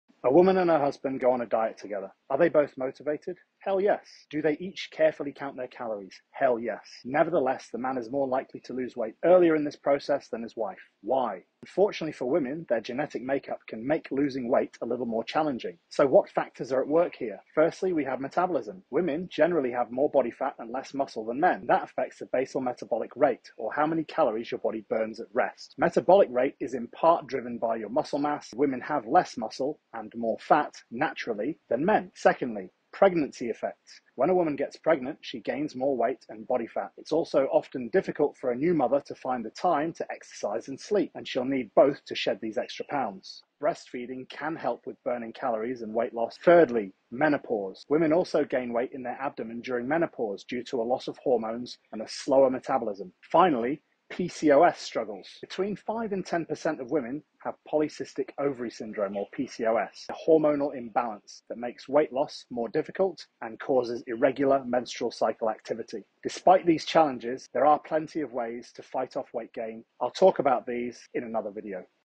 The Difference Between Men and Women
💯 As much as we may not want to believe it- women and men are not the same, and this needs to be taken into account when planning a diet and training program. Hormones and structural implications have to be considered.